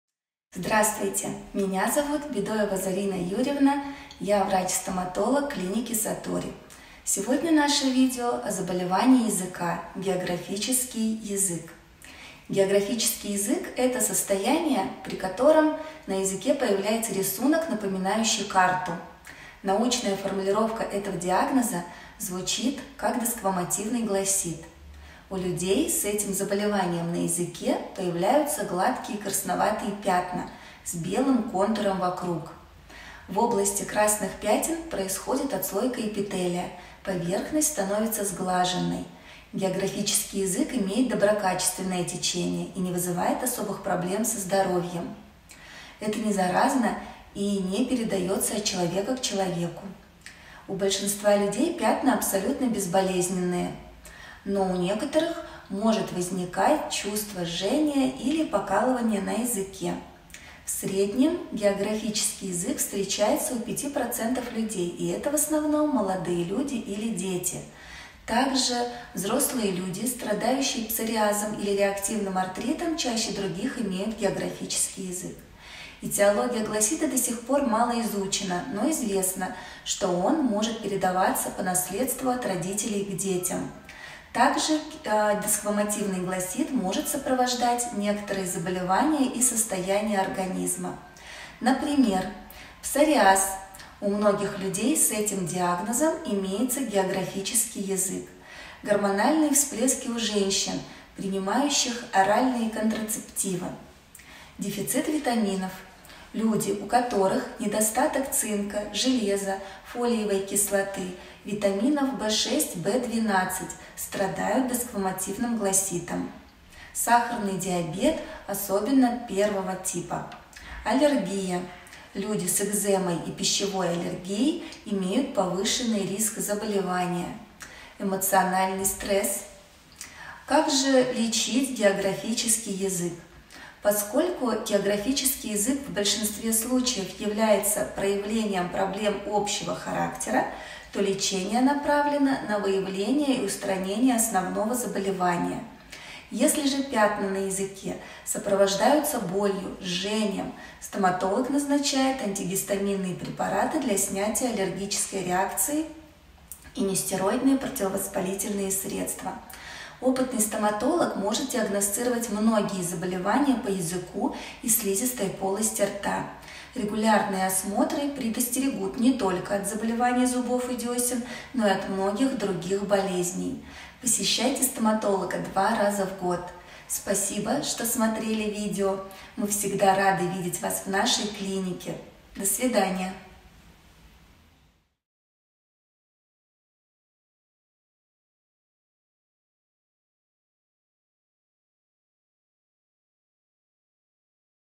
Как лечить географический язык?
В этом видео расскажем о заболевании языка - географический язык. Научная формулировка этого диагноза звучит как десквамативный глоссит.

Тайм-коды:
00:01 Географический язык - это..
01:16 Причины Глоссита
02:10 Как лечить географический язык